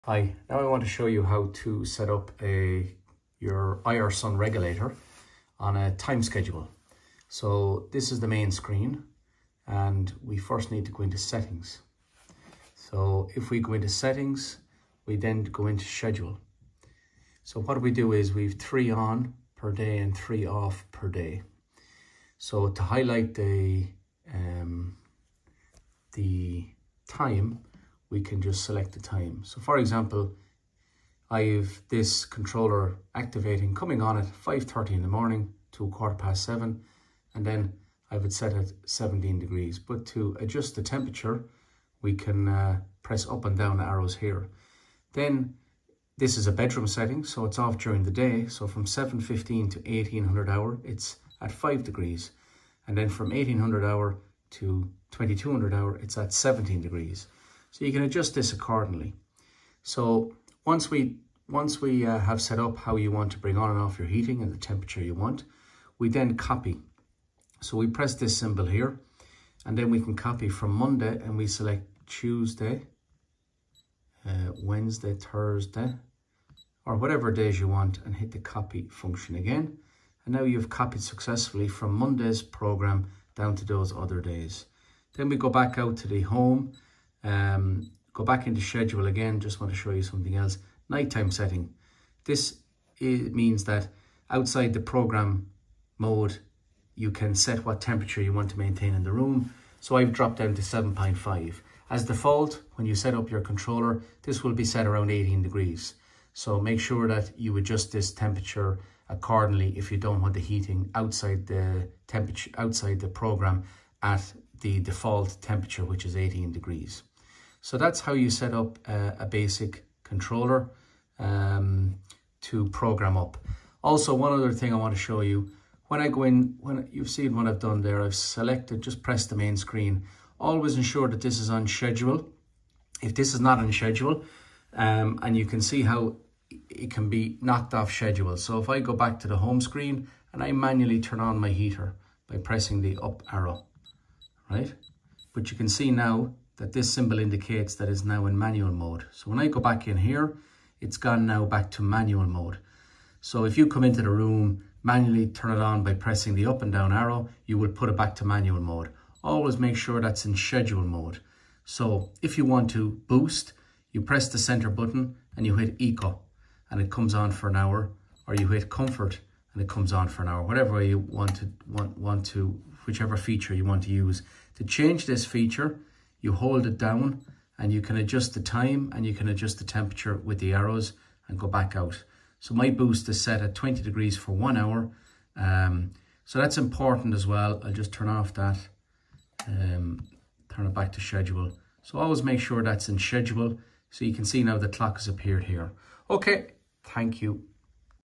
Set timer schedule on IR Sun Regulator controller
How to set up timber on the IR Sun Regulator Controller